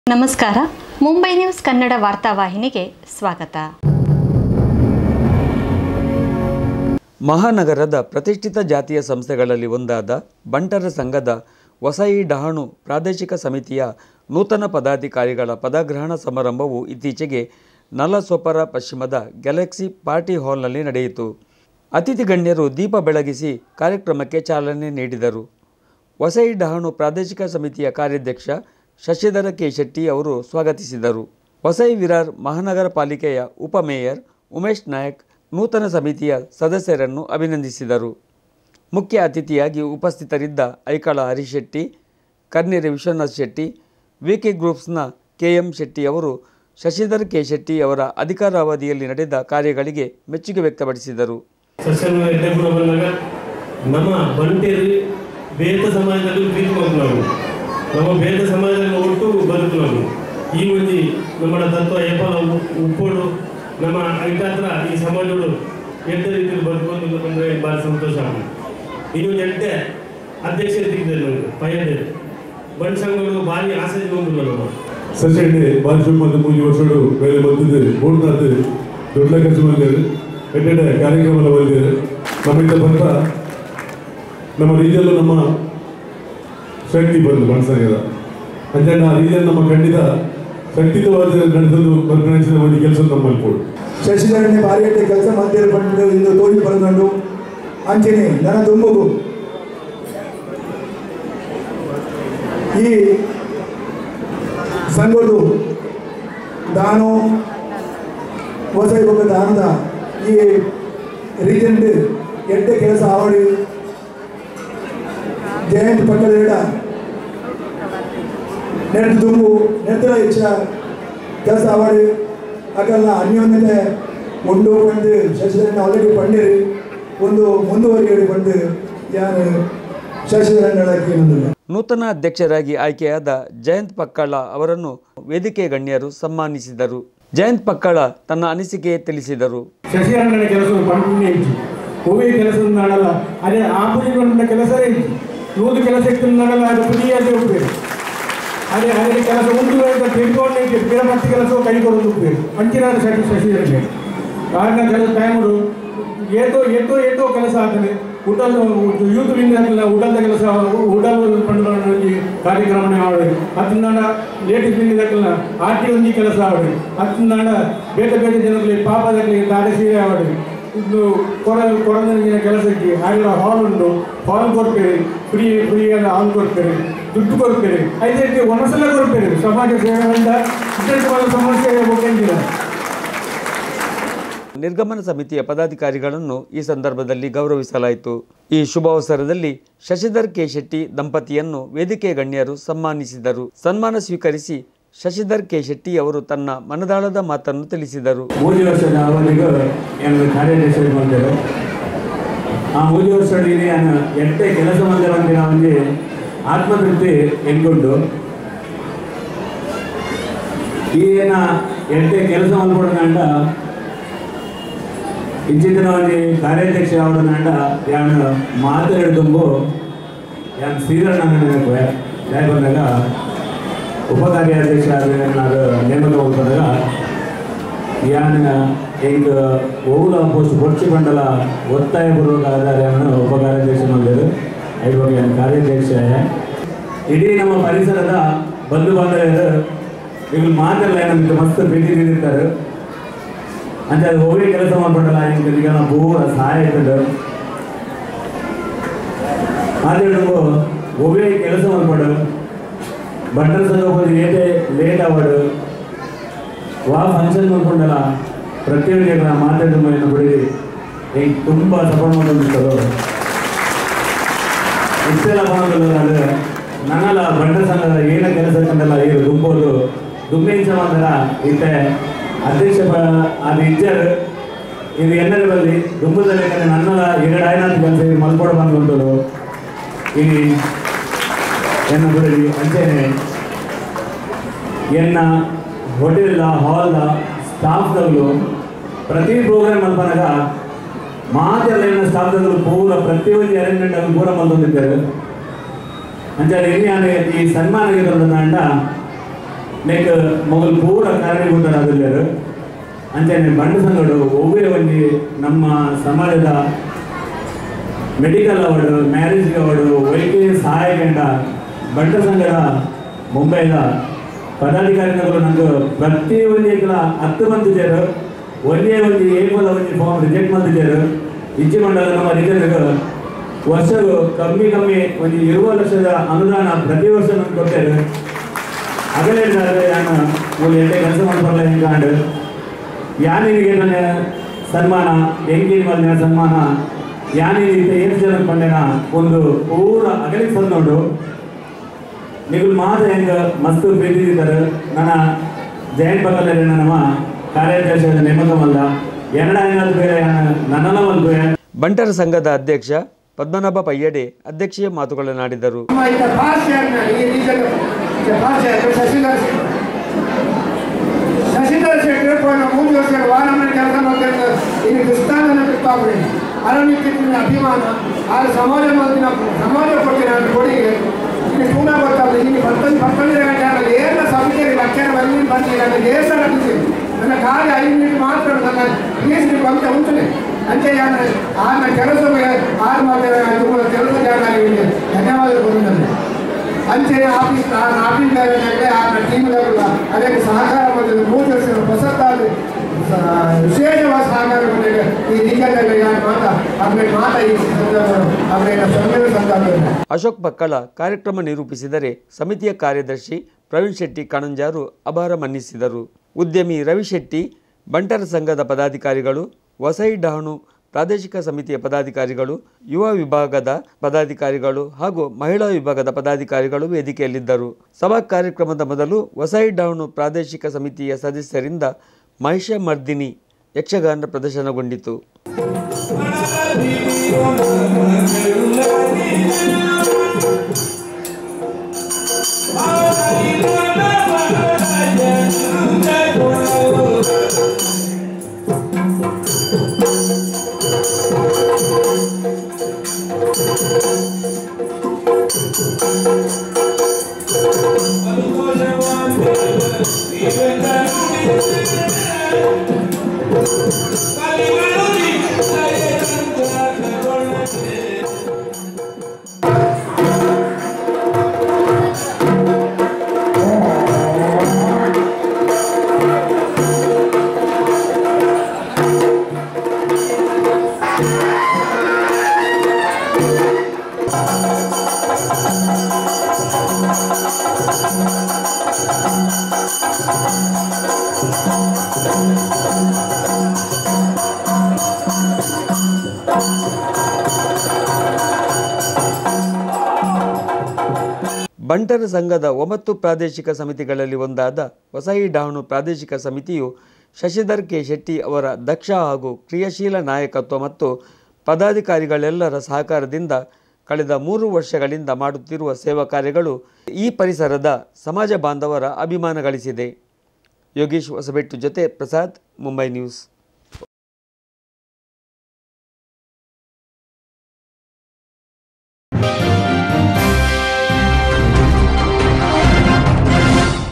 (Bunts Sangha Mumbai-Installation Ceremony of Vasai_Dahanu Samitis New Committee)
ಬಂಟರ ಸಂಘದ ವಸಾಯಿ ಡಹಾಣು ಸಮಿತಿಯ ನೂತನ ಪದಾಧಿಕಾರಿಗಳ ಪದಗ್ರಹಣ ಸಮಾರಂಭ.